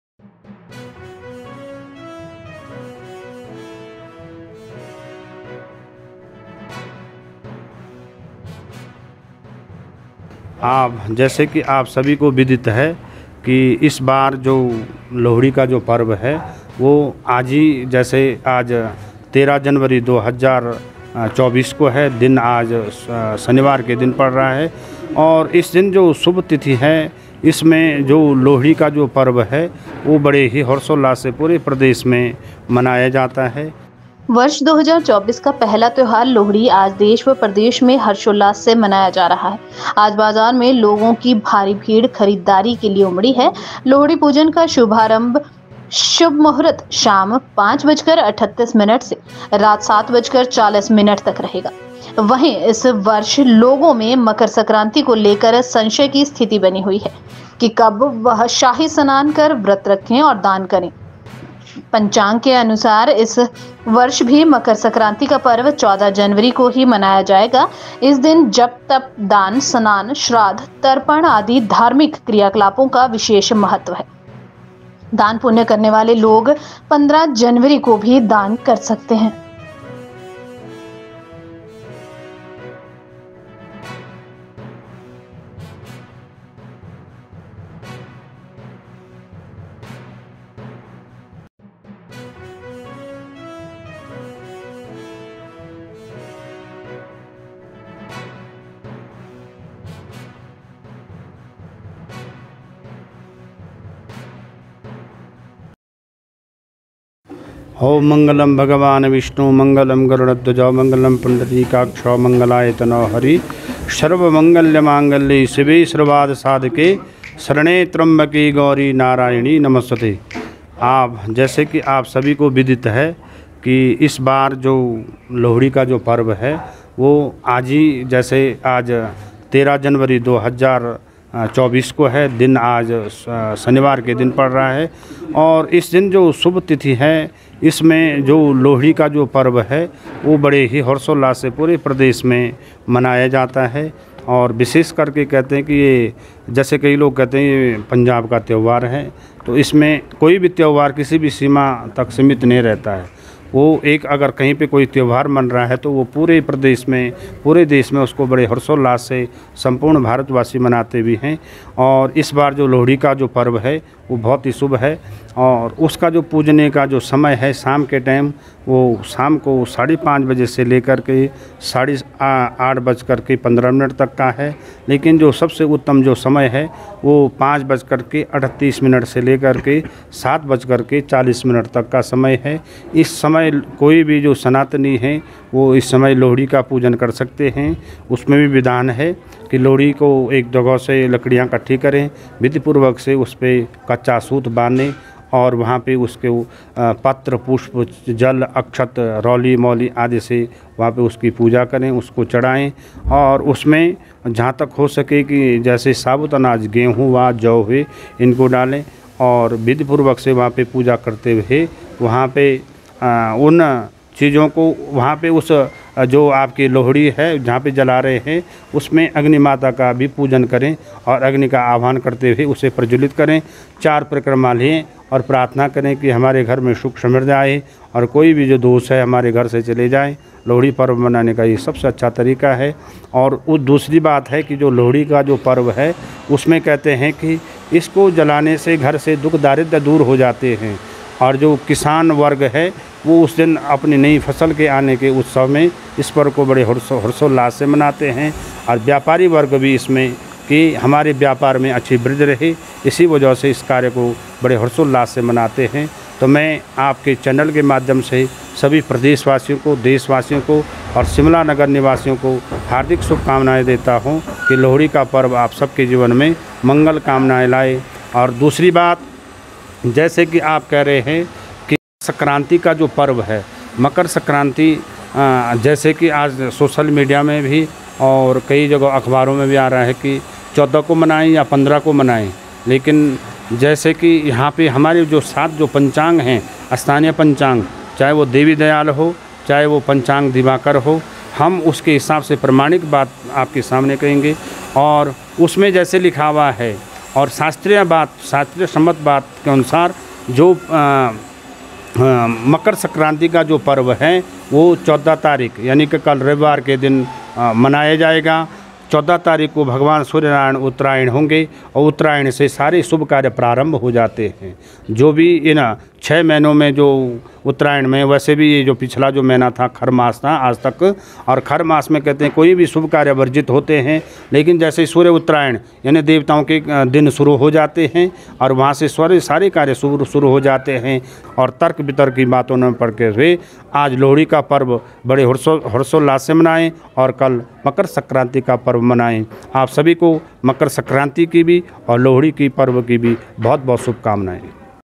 Lohri/Makar Sakranti/celebration
Lohri, the first festival of the year 2024, is being celebrated with great enthusiasm in the country and the state today. Today a huge crowd of people has gathered in the market for shopping. The auspicious time for Lohri puja will be from 5:38 pm to 7:40 pm.  This year, there is doubt among the people about Makar Sakranti as to when they should take a holy bath, fast and donate